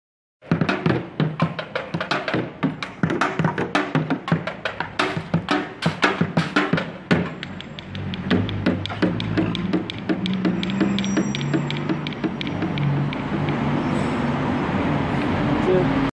Street drummer
Street drummer in downtown Bangkok